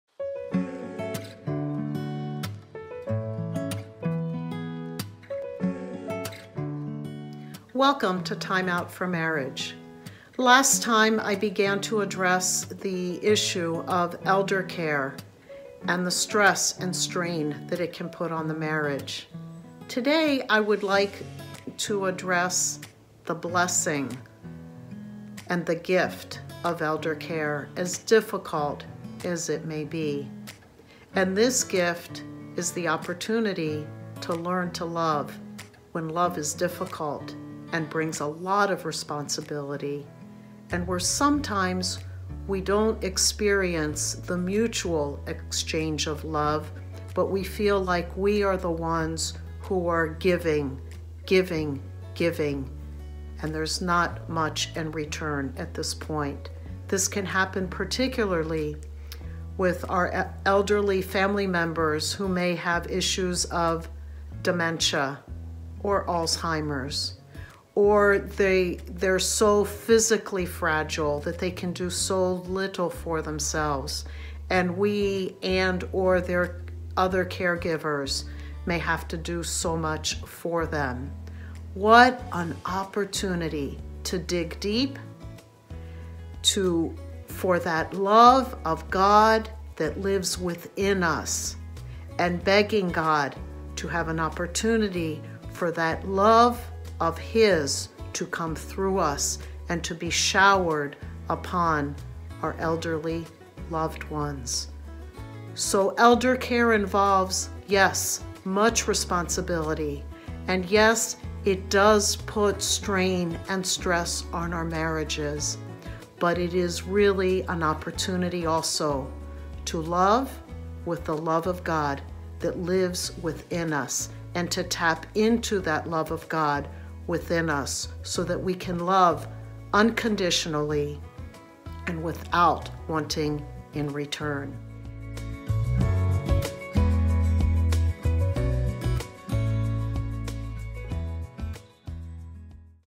Time Out for Marriage: Elder Care Pt. 2
In this episode of "Time Out for Marriage" from the GOA Center for Family Care, Pres. Kerry Pappas continues with her discussion of elder care and the surprising blessings that can come with it.